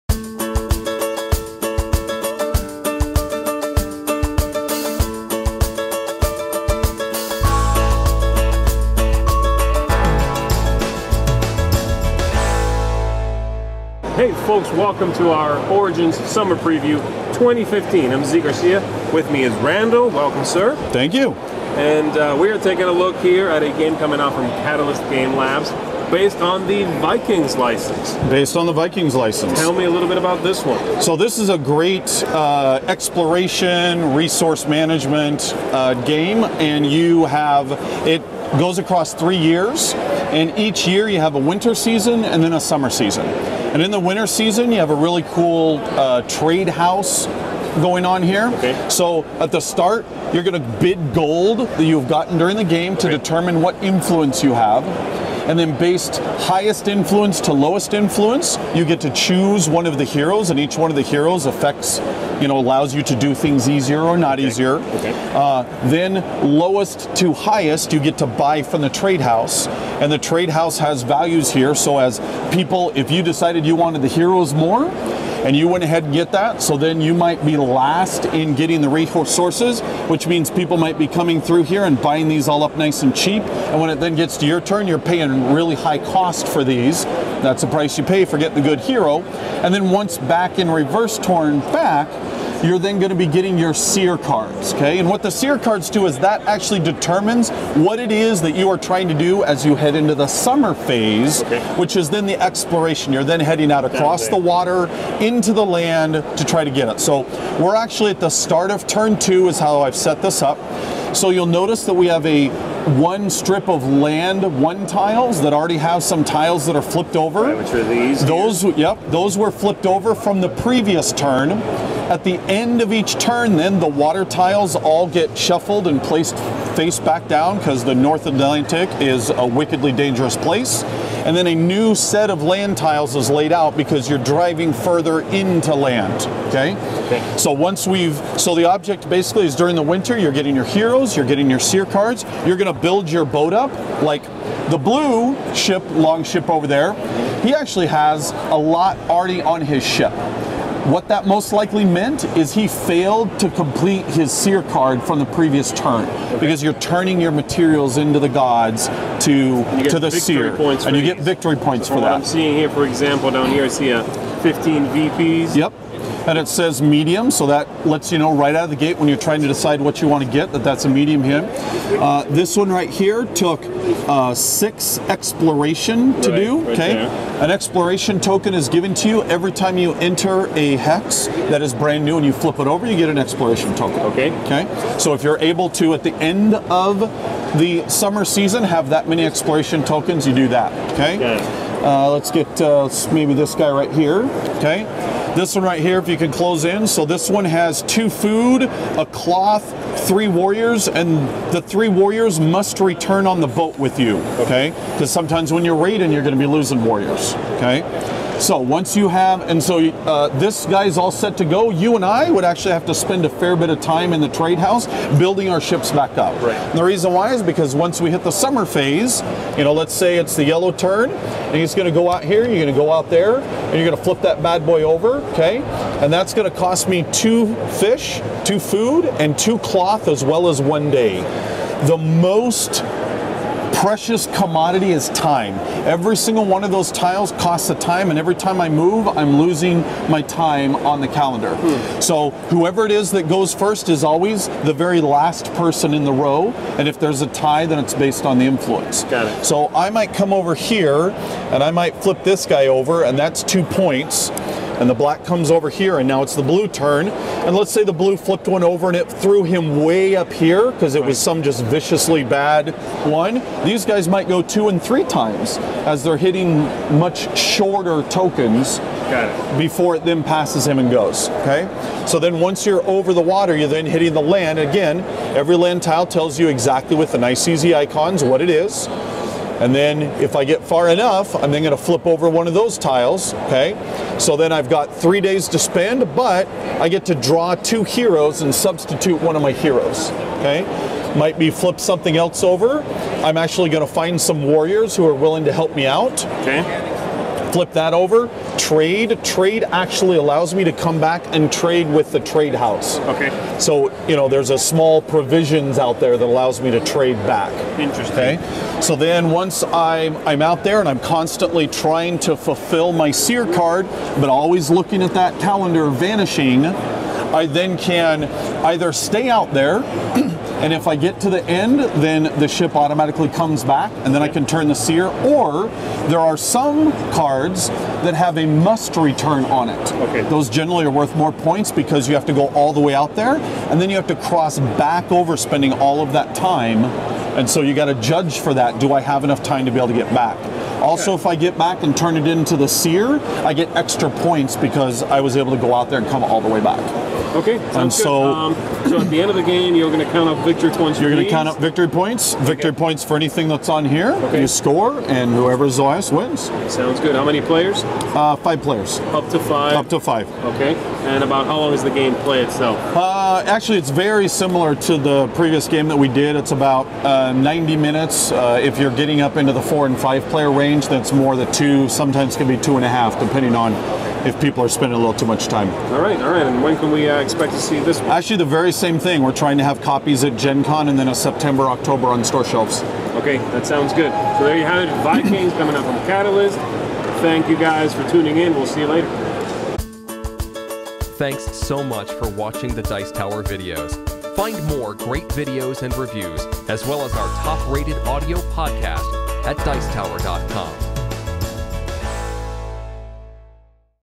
Origins Summer Preview: Vikings Board Game
The Dice Tower talks to Catalyst Games about their upcoming game based on the popular TV series